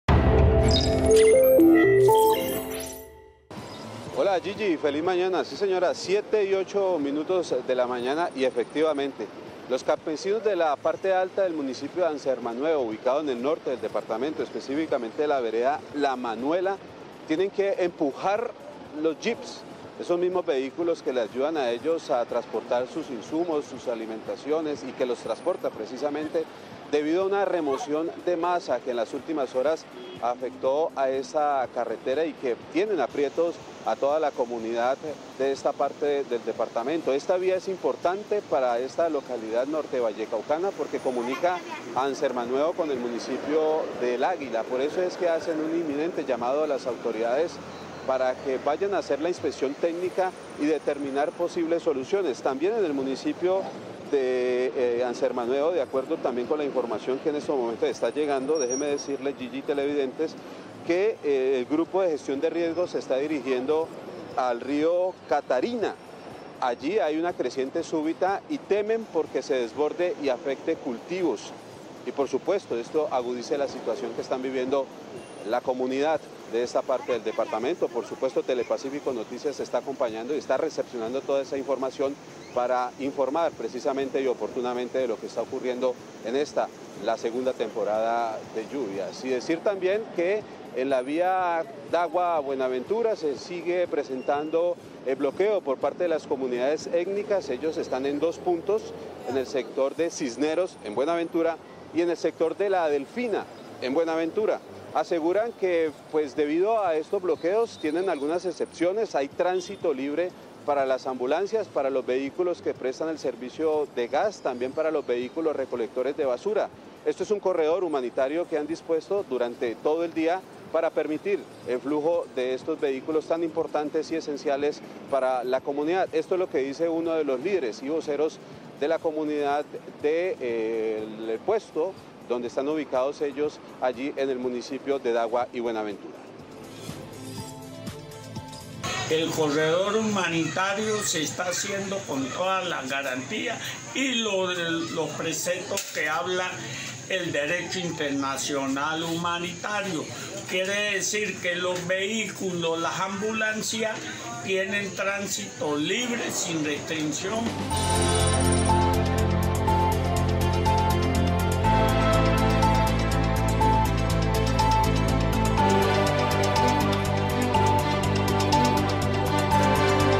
Mal estado de la vía entre Andalucía y El Águila por cuenta de las lluvias | 23.11.22 | TP Noticias
Valle | Un corredor entre Andalucía y El Águila está gravemente afectado por cuenta de las fuertes precipitaciones de los últimos días.